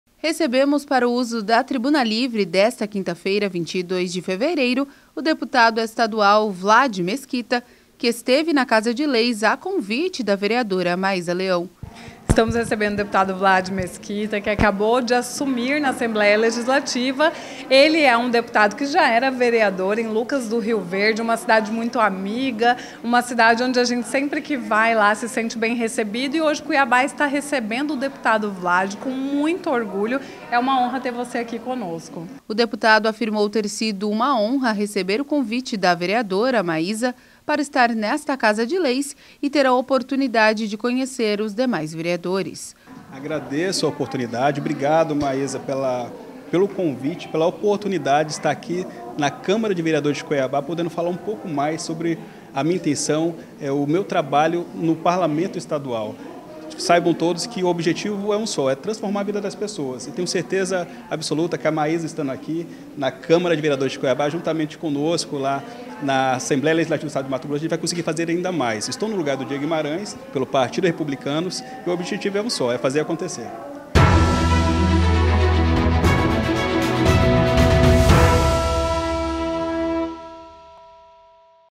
22.02.2024 - DEPUTADO ESTADUAL VLAD MESQUITA PARTICIPA DE TRIBUNA LIVRE
RECEBEMOS PARA O USO DA TRIBUNA LIVRE DESTA QUINTA-FEIRA, 22 DE FEVEREIRO, O DEPUTADO ESTADUAL VLAD MESQUITA, QUE ESTEVE NA CASA DE LEIS A CONVITE DA VEREADORA MAYSA LEÃO.